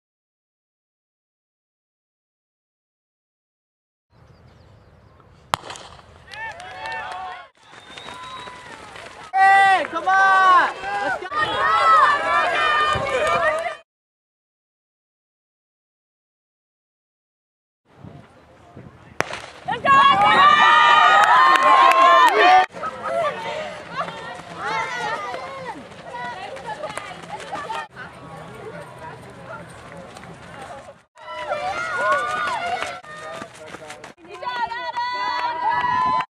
Dominican Cross Country Highlights - October 15, 2011
From the 2011 Bronco Invitational featuring runners Ally Rosemond and Adam Scott.